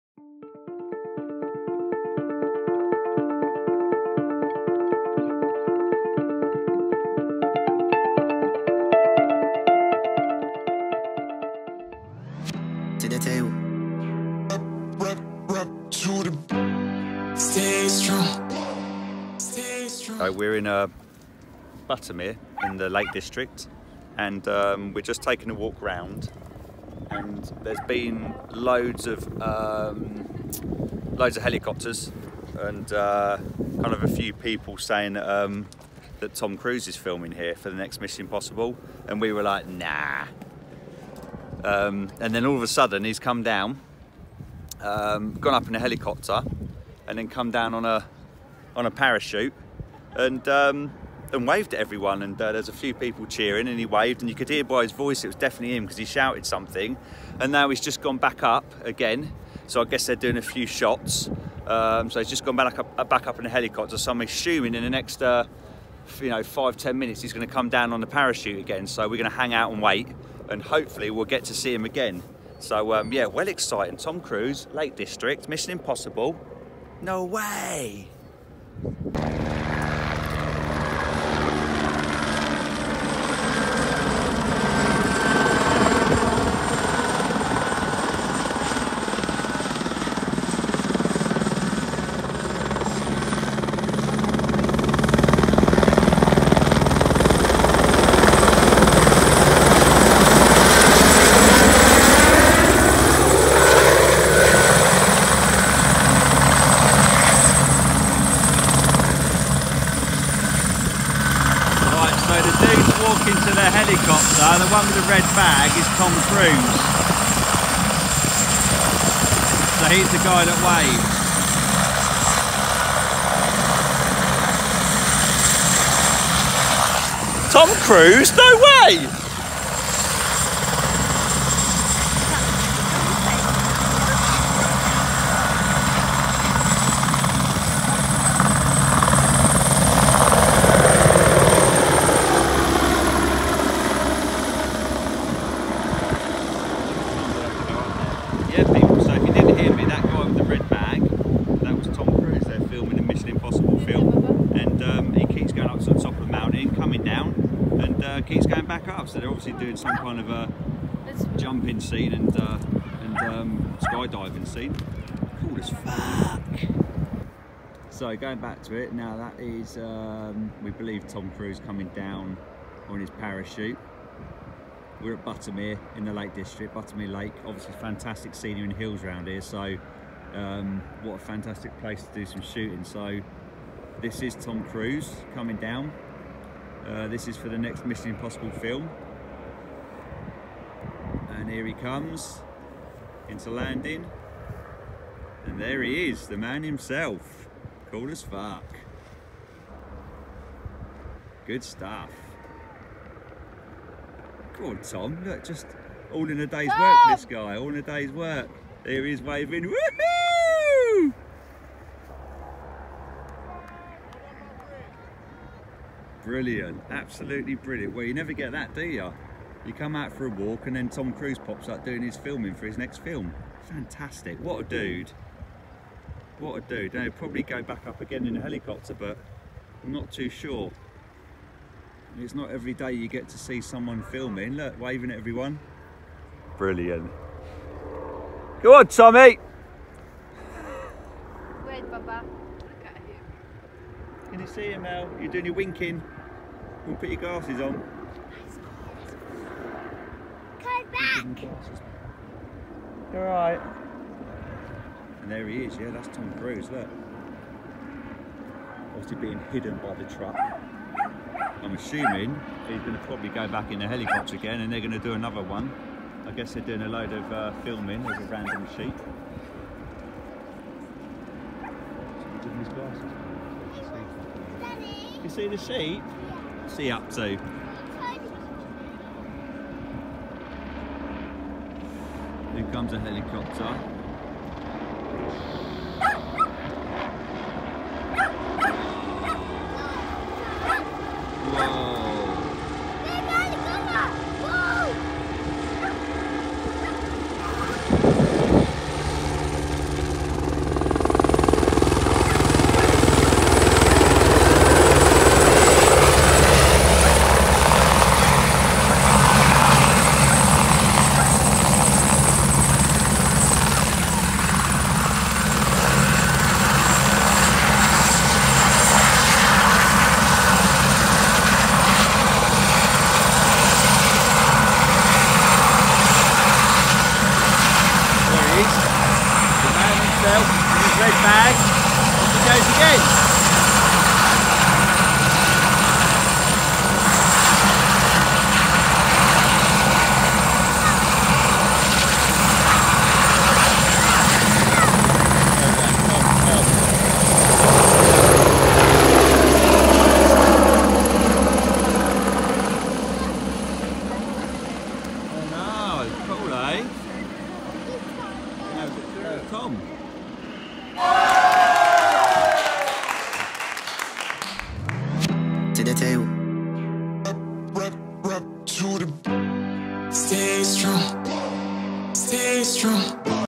☯️ TOM CRUISE -Buttermere (Lake District) Filming the NEW Mission Impossible FILM! 😱
Watch ET USA ( Entertainment Tonight ) preview !